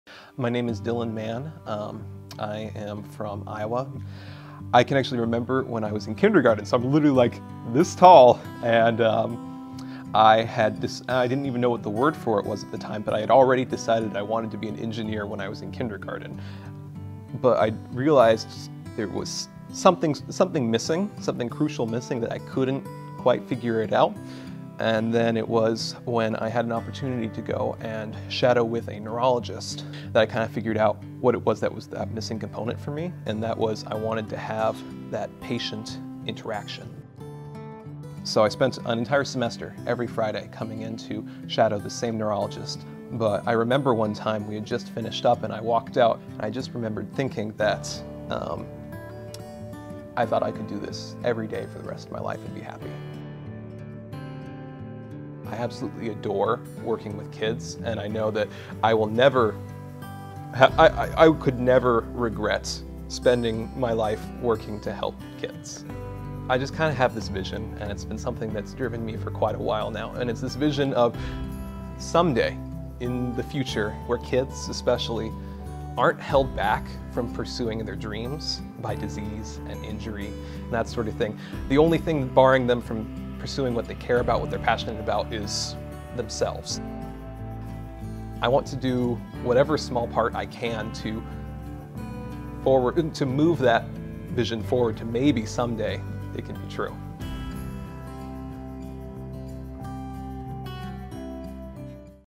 Carle Illinois College of Medicine Student Profile | Dylan Mann
Dylan Mann, one member of the Carle Illinois inaugural class, shares his story about his journey to medical school.